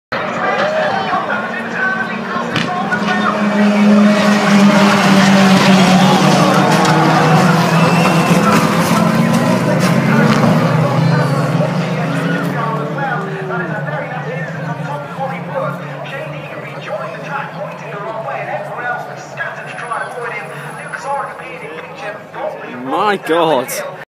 Mayhem at donington race track with Renault Clio's crashing at the first corner!!
So I went to donington race circuit on the 15/16 of april and this was one of the few crashes i caught on the stage